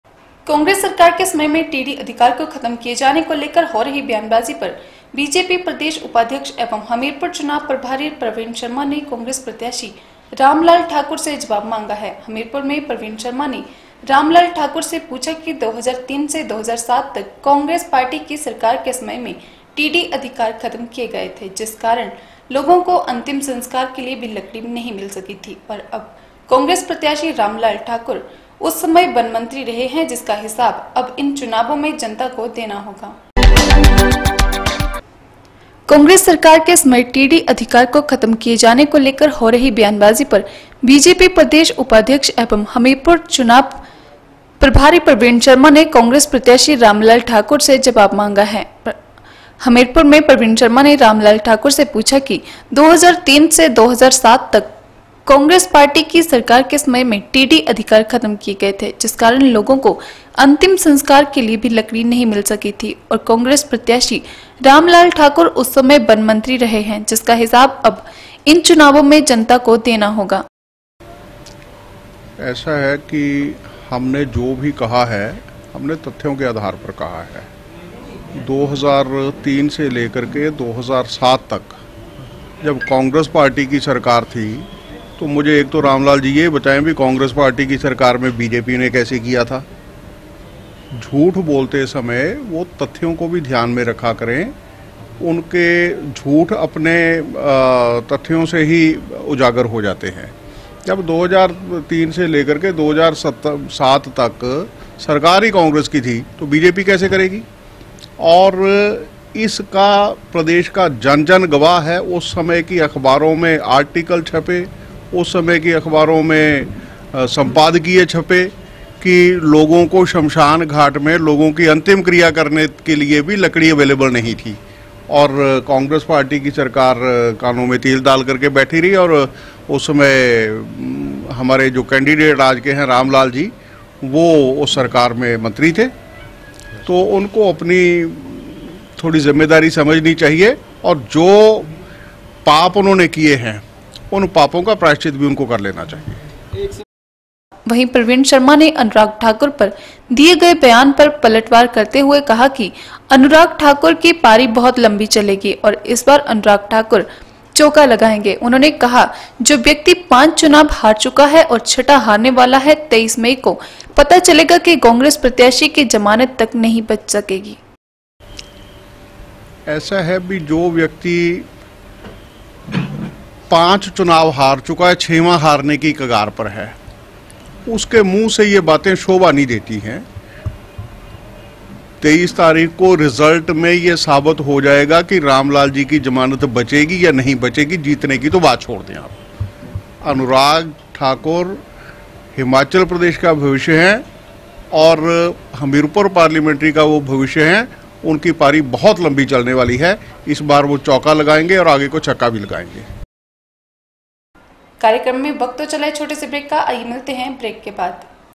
8 MAY N 2 B 1Praveen Sharma overturned TD rights demanded from Congress candidate Ramlal Thakur
कांग्रेस सरकार के समय टीडी अधिकार को खत्म किए जाने को लेकर हो रही बयानबाजी पर बीजेपी प्रदेश उपाध्यक्ष एवं हमीरपुर चुनाव प्रभारी प्रवीण शर्मा ने कांग्रेस प्रत्याशी रामलाल ठाकुर से जबाव मांगा है।